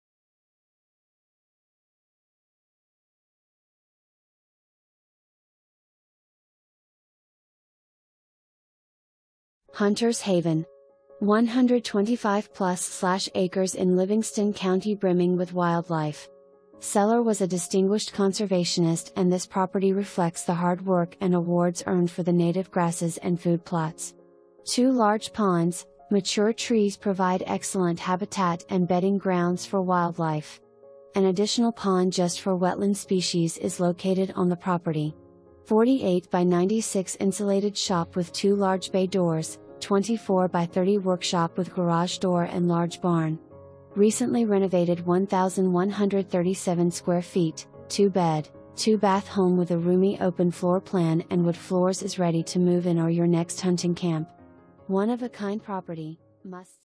1780 US Hwy 60 E Burna KY 42028
HUNTER’S HAVEN! 125 +/- Acres in Livingston County brimming with wildlife. Seller was a distinguished conservationist and this property reflects the hard work & awards earned for the native grasses and food plots. Two large ponds, mature trees provide excellent habitat and bedding grounds for wildlife. An additional pond just for wetland species is located on the property. 48x96 insulated shop with 2 large bay doors, 24x30 workshop with garage door and large barn. Recently renovated 1137 sqft, 2 bed, 2 bath home with a roomy open floor plan and wood floors is ready to move in or your next hunting camp. One of a kind property, must see!

Jessica & Kevin Housman
Housman Partners Real Estate